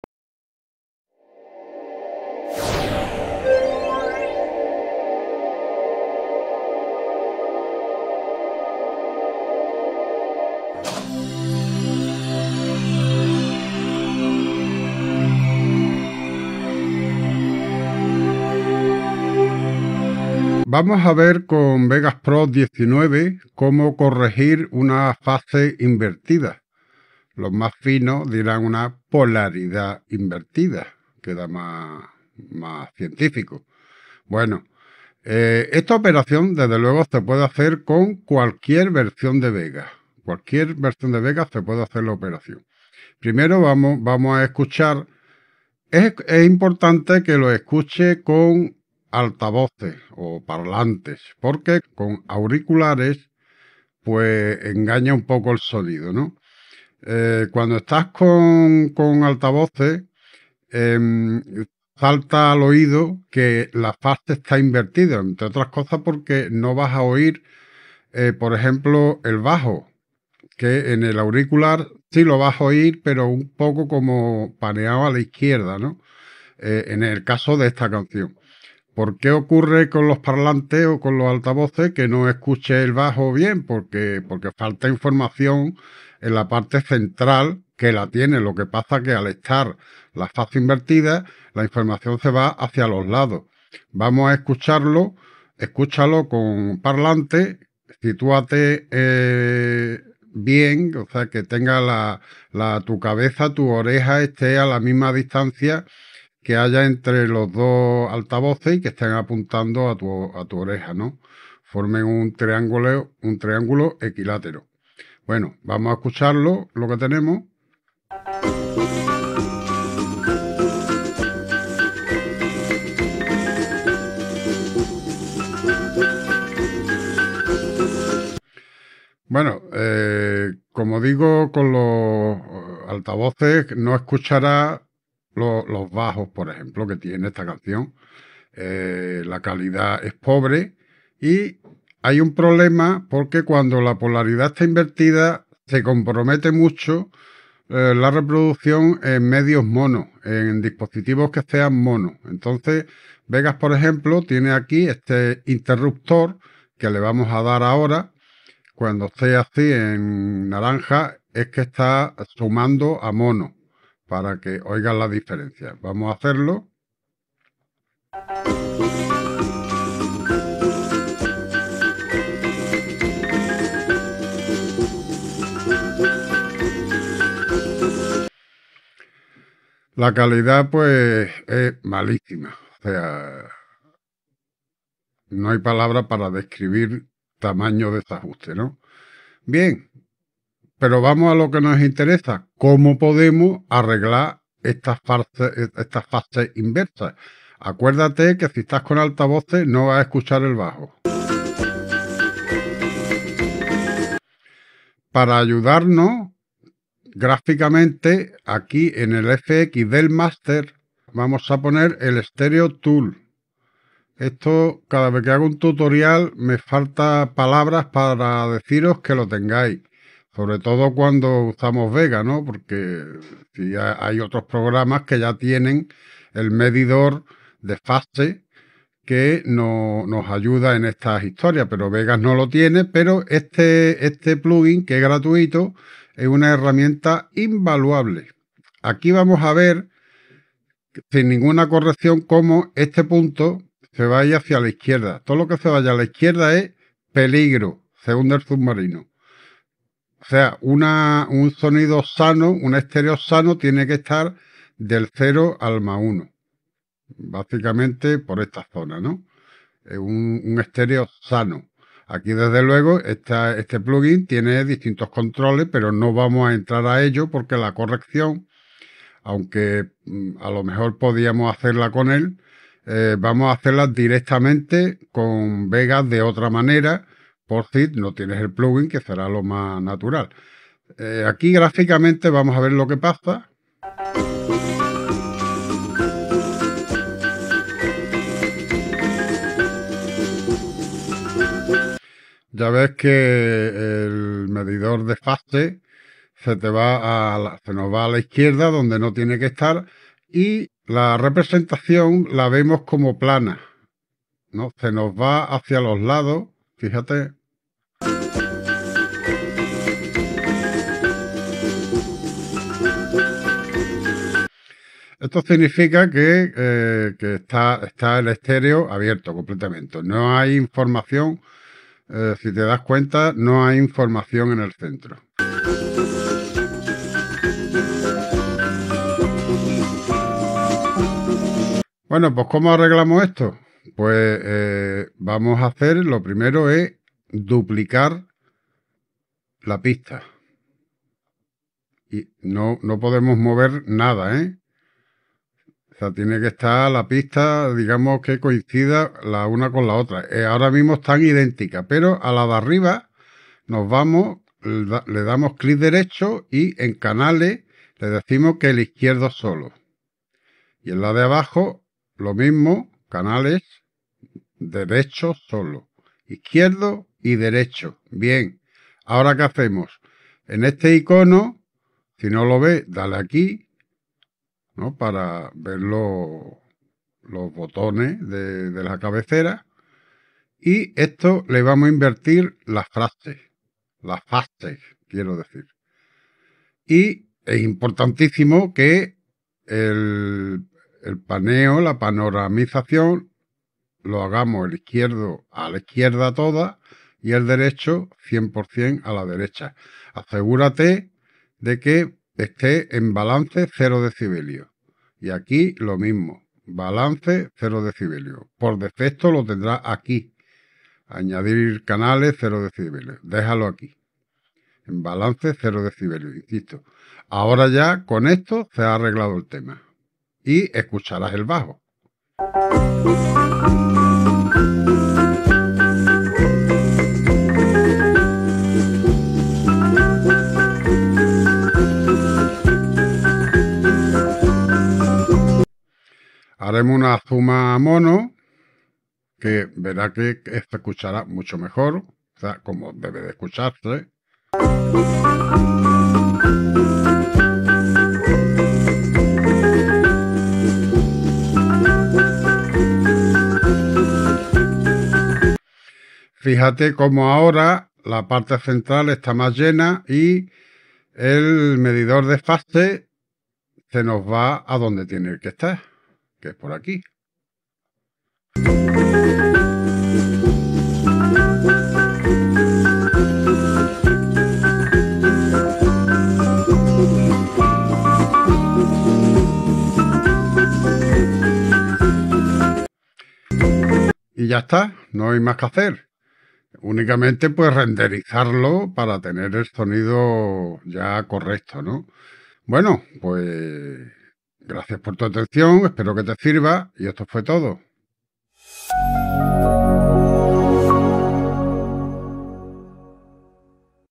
Vegas Pro Audio: Corrección de fase invertida.
Una fase invertida no es problema con Vegas Pro.